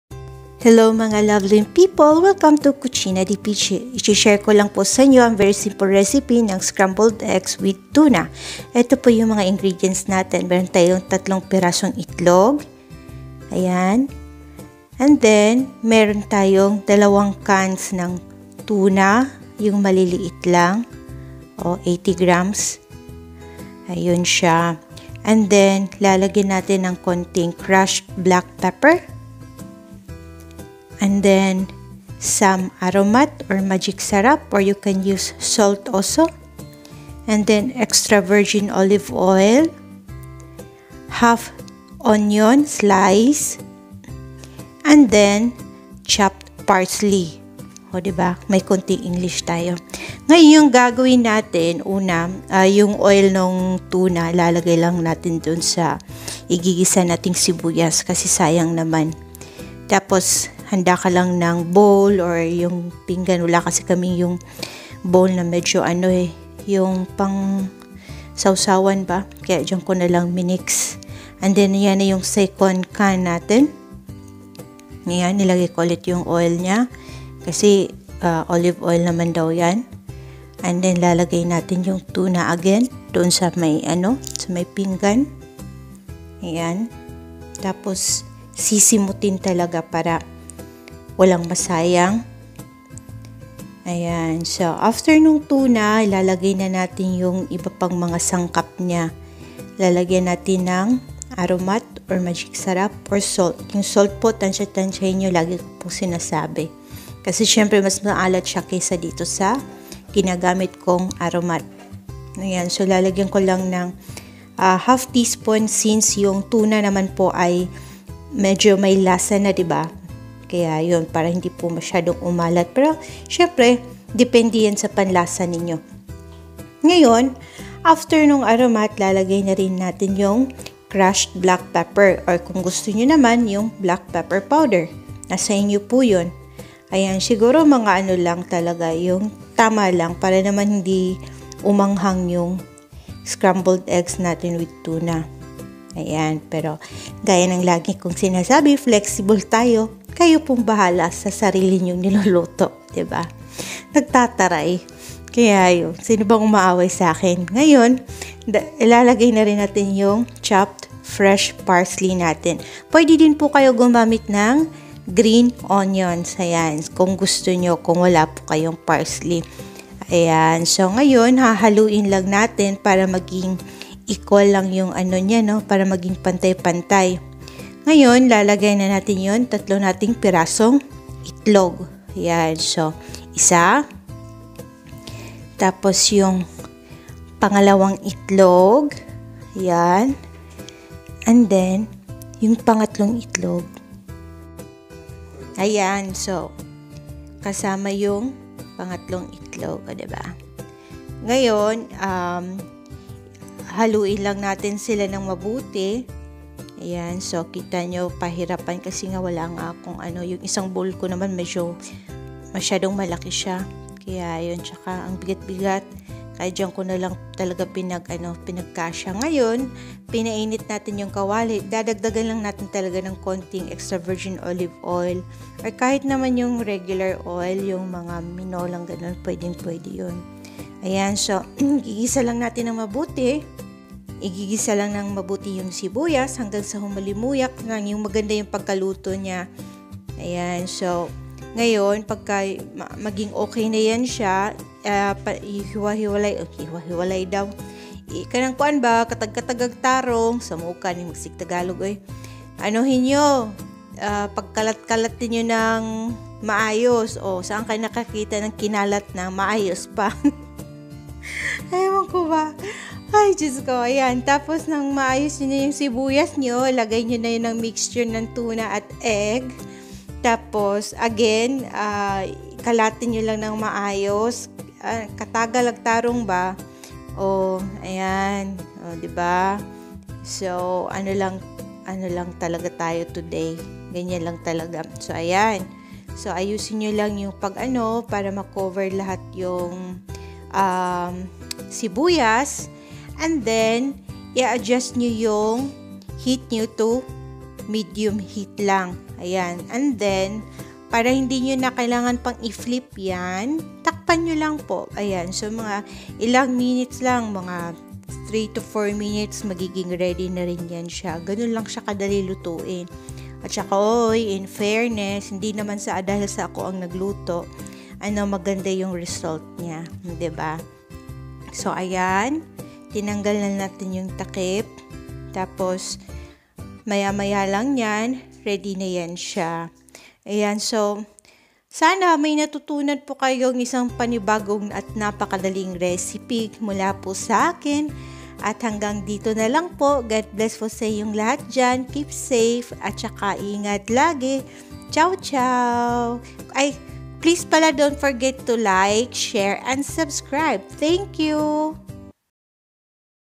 Scrambled Eggs with Tuna
Another very simple recipe po. You can use green onions po if you don't have parsley.